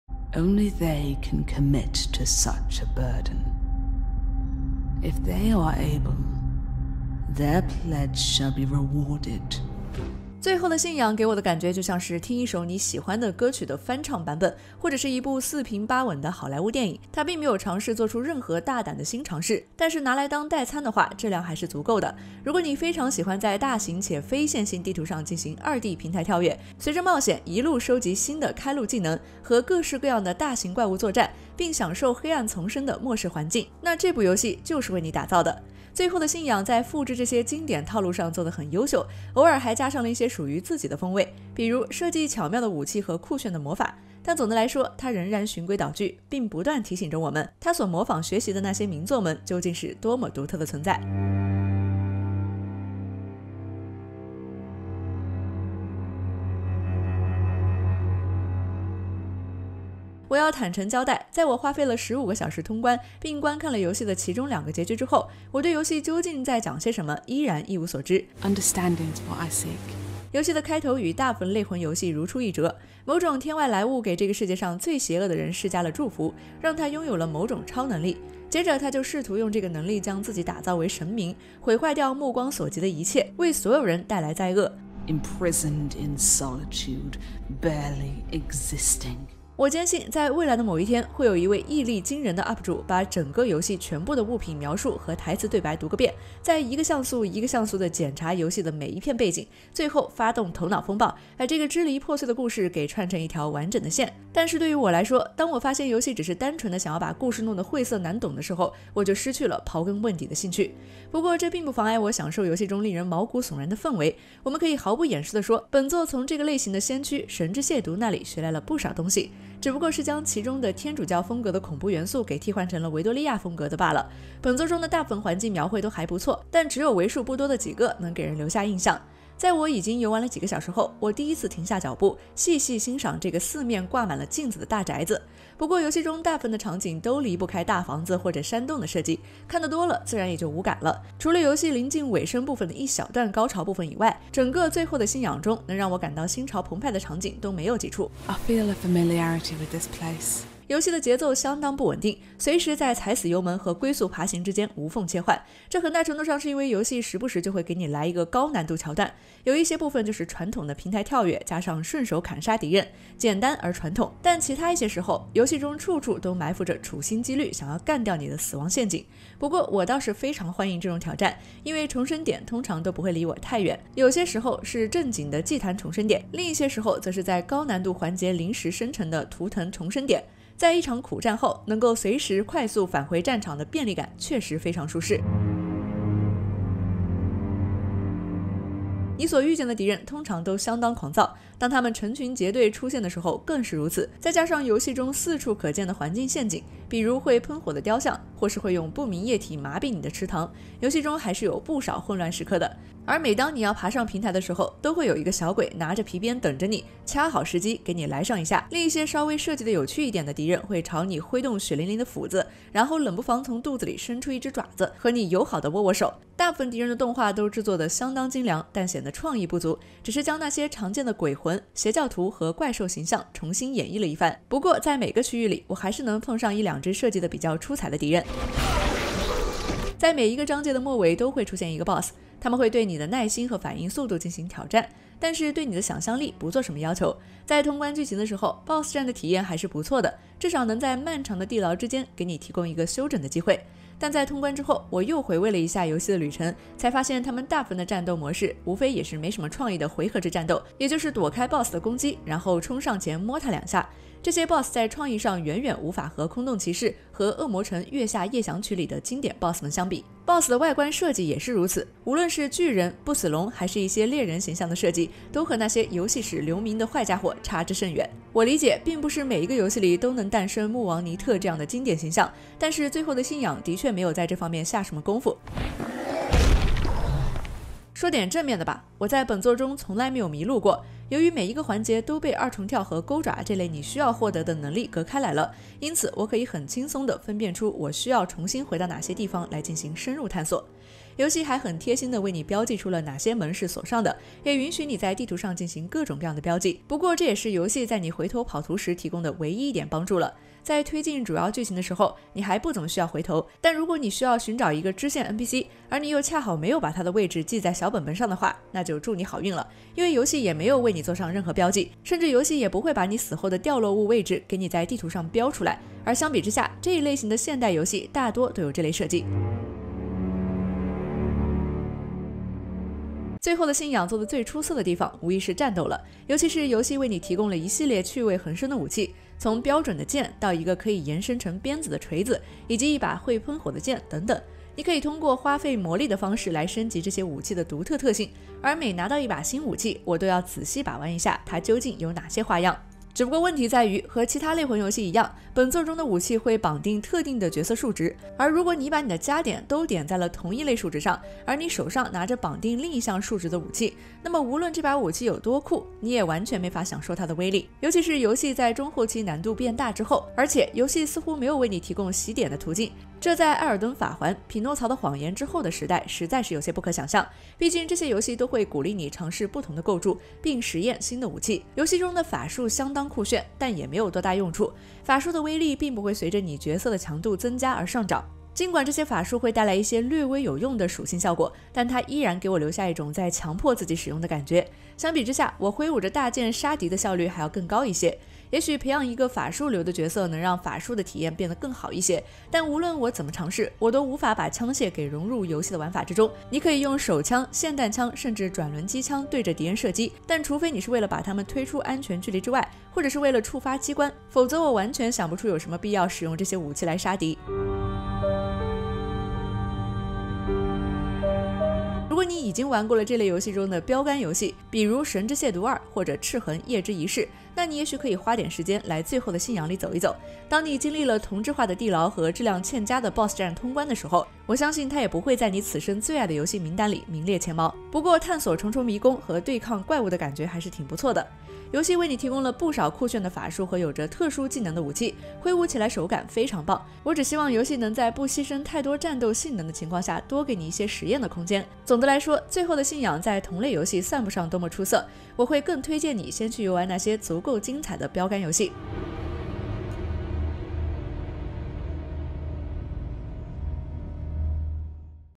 《最後的信仰》遊戲評測 The Last Faith Review
《最後的信仰》在復制這些經典套路上做的很優秀，偶爾還加上了一些屬於自己的風味，比如設計巧妙的武器和炫酷的魔法。但是總的來說，它仍然循規蹈矩，並不斷提醒著我們，它所模仿學習的那些名作們究竟是多麼獨特的存在。